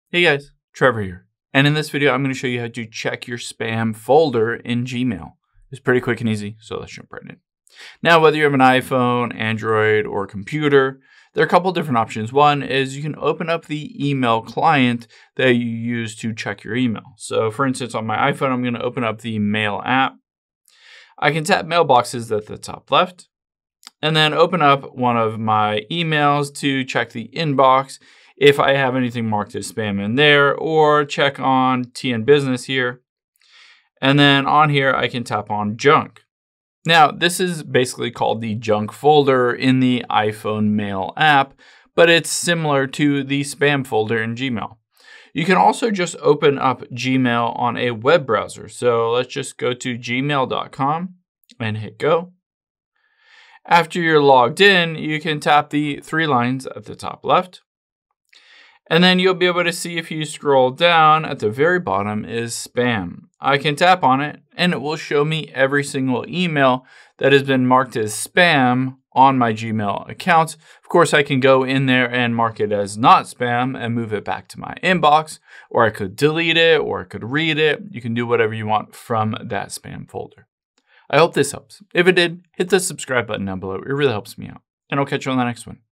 How To Check Spam Folder In Gmail
Here's how to check the spam folder in Gmail so you can see what emails have been automatically filtered to keep your inbox clear.